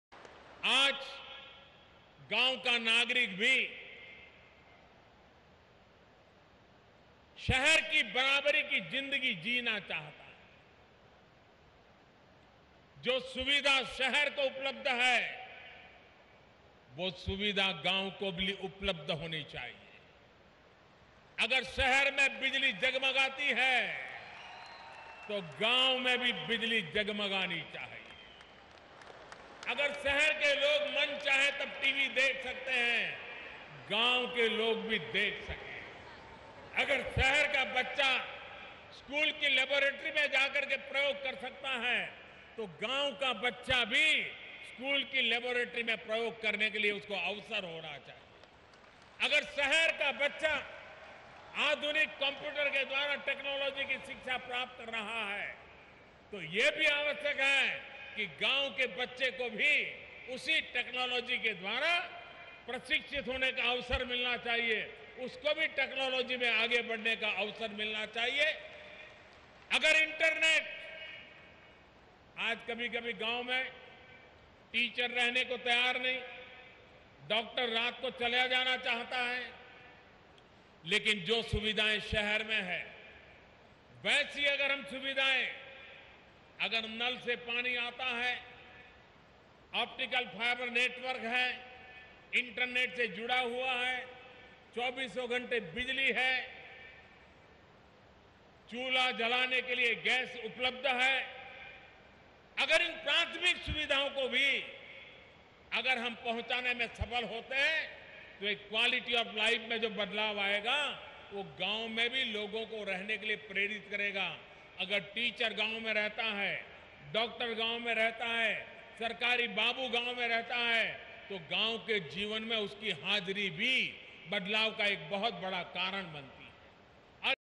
Facilities available in urban areas should also be present in our villages
Prime Minister Narendra Modi at Birth Centenary Celebration of Nanaji Deshmukh at IARI, New Delhi